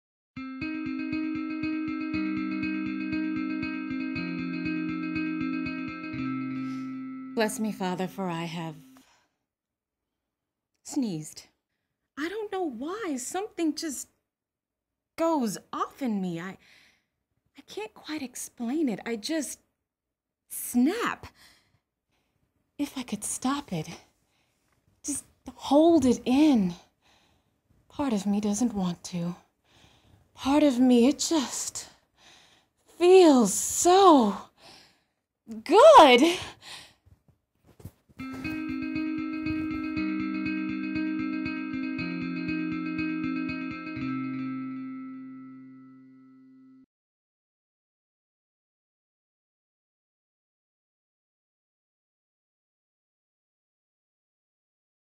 "Bless Me Father" by Boris Khaykin
"Bless Me Father" was one of five winners of Project Y Theatre Company's Monologue Contest in May 2010. The Top 5 videos were shown at Project Y's 2nd Annual Fundraiser Party in New York City. For more information about Project Y, please